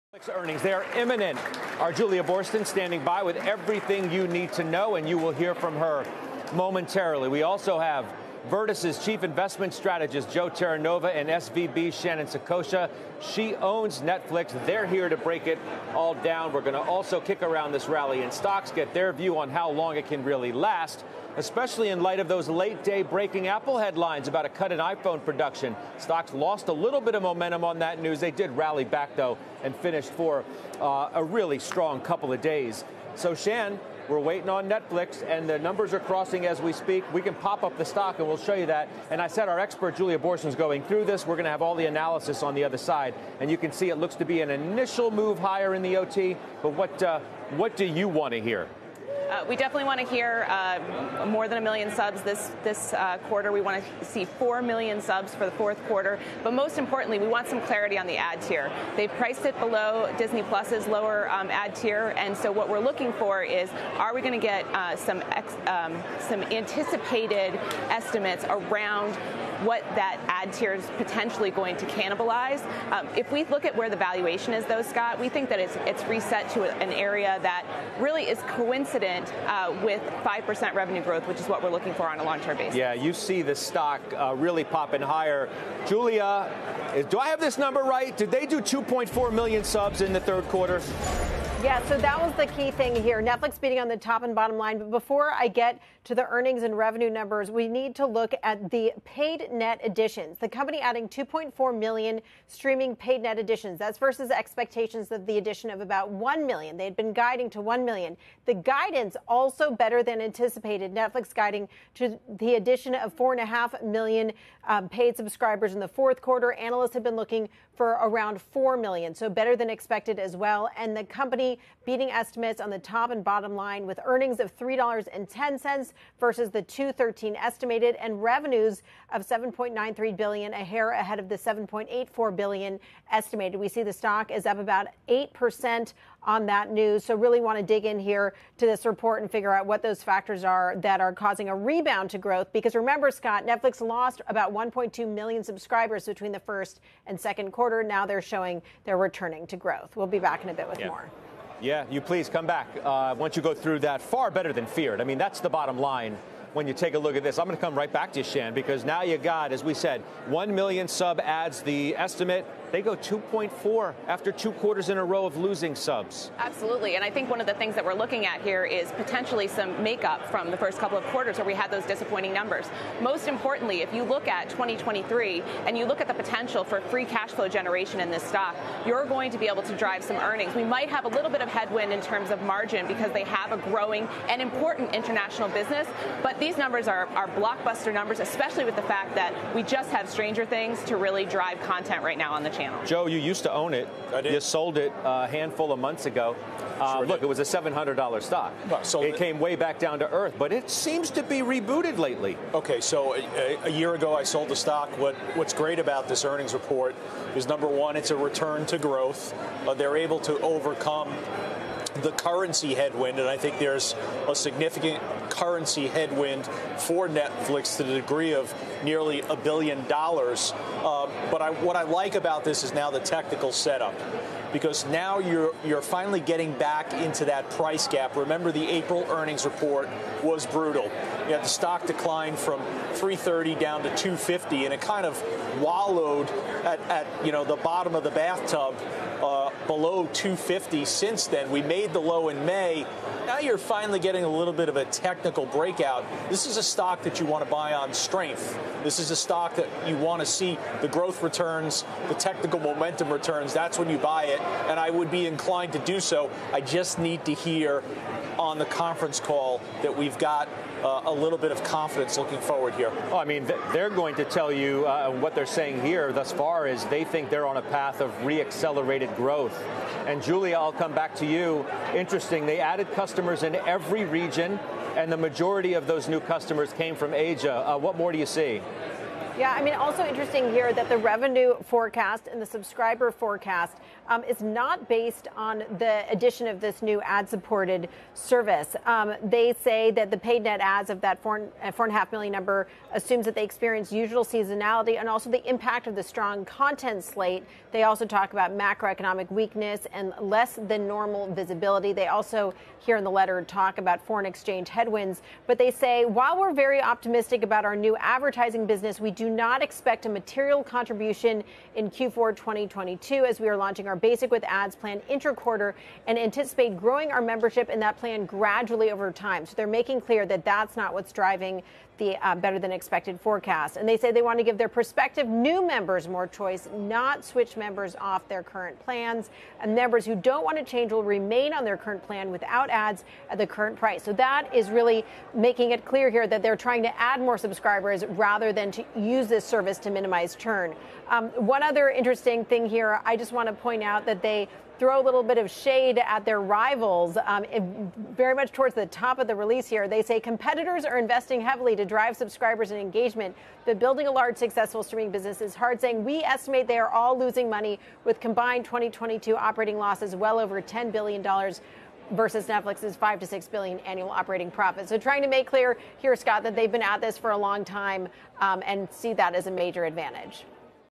Netflix beats on top and bottom, adds 2.4M subs vs. 1.09M estimated
Virtus Investment Partners' Joe Terranova and SVB Private's Shannon Saccocia join 'Closing Bell: Overtime' to discuss Netflix earnings and just how long the rally can last. With CNBC's Julia Boorstin.